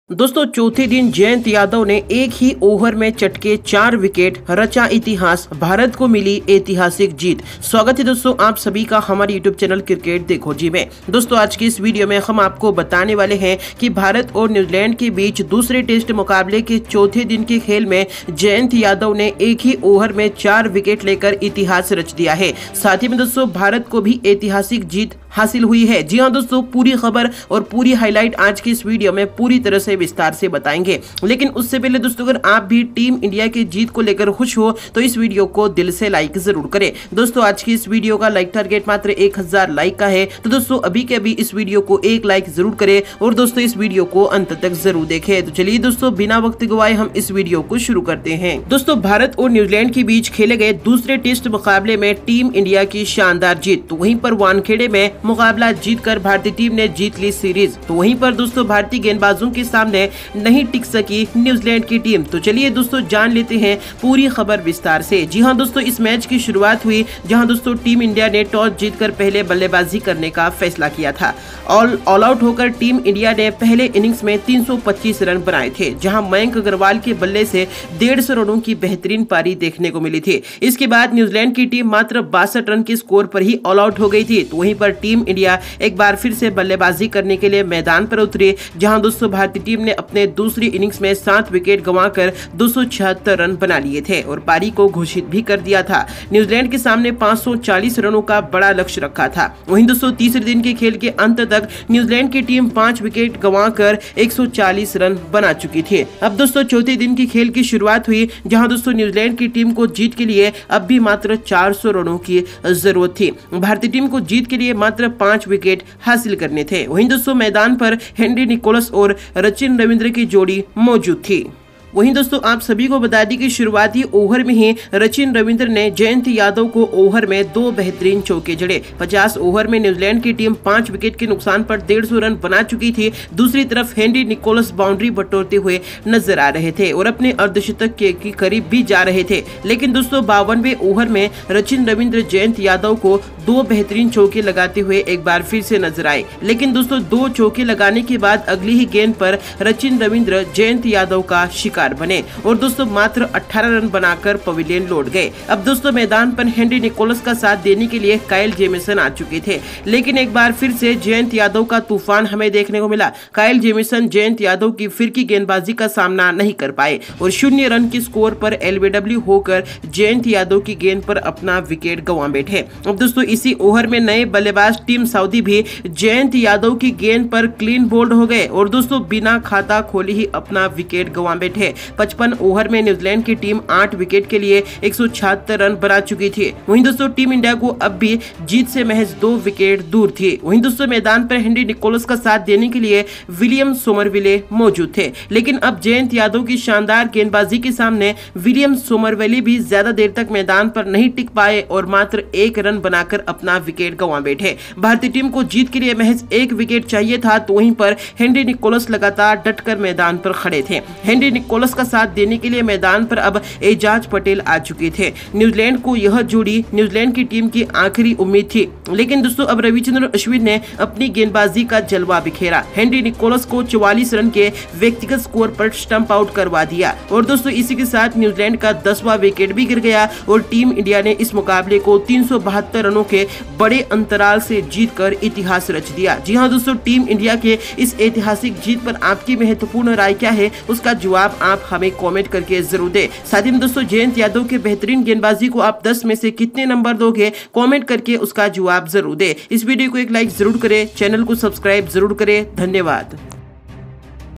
India vs Newzealand • IND vs NZ 2nd Test DAY 4 Full Highlights •Today Match Highlights,Jayant Yadav
India vs Newzealand • India vs Newzealand • IND vs NZ 2nd Test DAY 4 Full Highlights •Today Match Highlights, Jayant Yadav,Ashwin

In this video

India’s Test squad against New Zealand: Ajinkya Rahane (Captain)• KL Rahul• Mayank Agarwal• Cheteshwar Pujara (vice-captain)• Shubman Gill• Shreyas Iyer• Wriddhiman Saha (wicket-keeper)• KS Bharat (wicket-keeper)• Ravindra Jadeja• R. Ashwin• Axar Patel• Jayant Yadav• Ishant Sharma• Umesh Yadav• Md. Siraj• Prasidh Krishna.